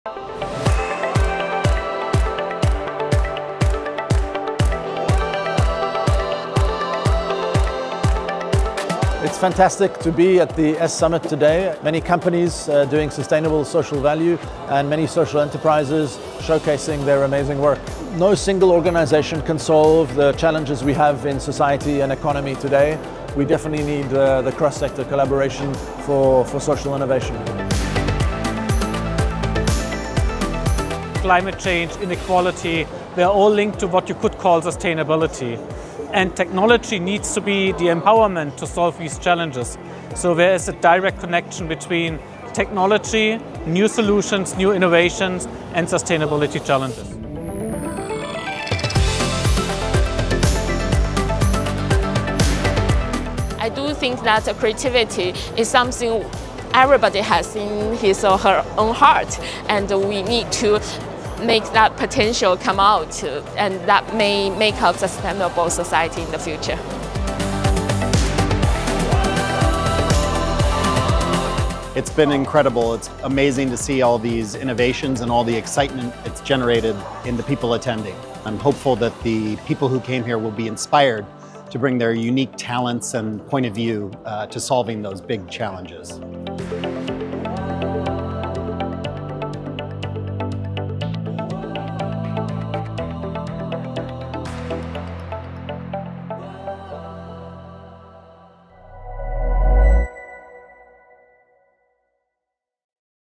Building a Sustainable Future: AI, Innovation, and Collaboration at S Summit 2025
Sustainable Social Value Innovation Summit 2025 brought together leaders from academia, businesses, social enterprises, non-profits, and the UN — not to talk about problems, but to build solutions.

AI that supports mental health. Tools that protect communities from extreme weather. Youth creators rewriting what innovation looks like today. New models for impact investing.

And yes — a fashion show featuring clothes made from captured CO₂.

This is what happens when collaboration is integrated into sustainable development.

👉 Dive into the highlights and feel the energy of a future being built right now.